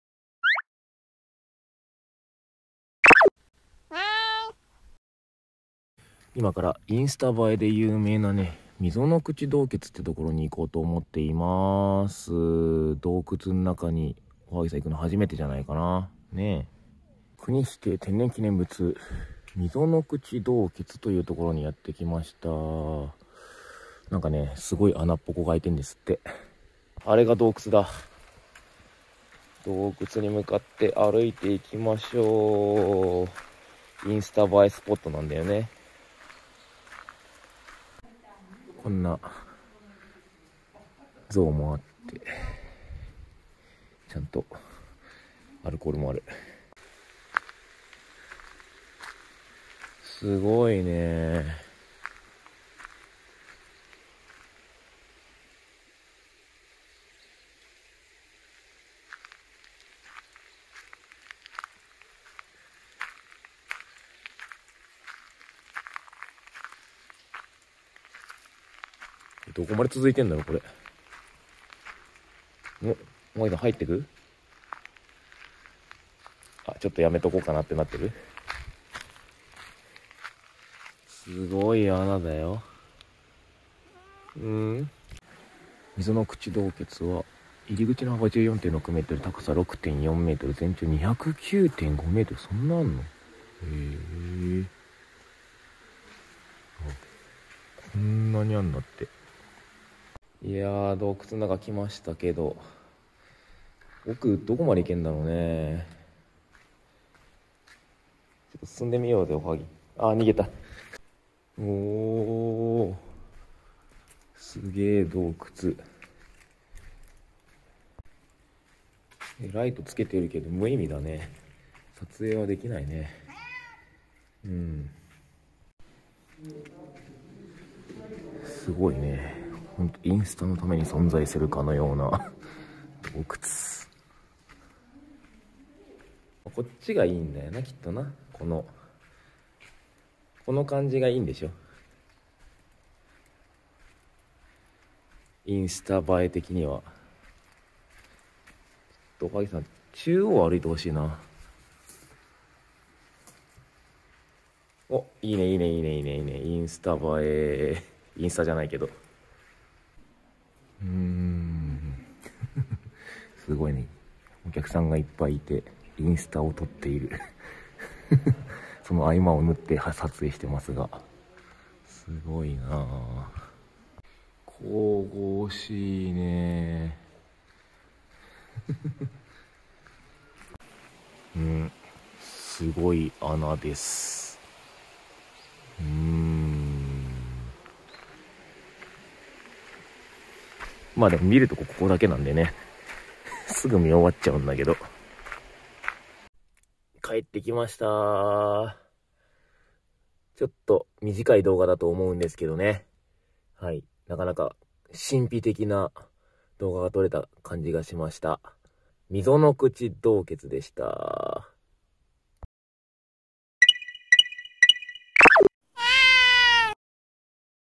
【猫と旅行】溝ノ口洞穴は映えスポット【鹿児島県】
猫と車中泊しながら旅行しています

見所は少ないですが、なるほど映えるなぁ、と感心しきりでした

最新の目的地はTwitterで報告しています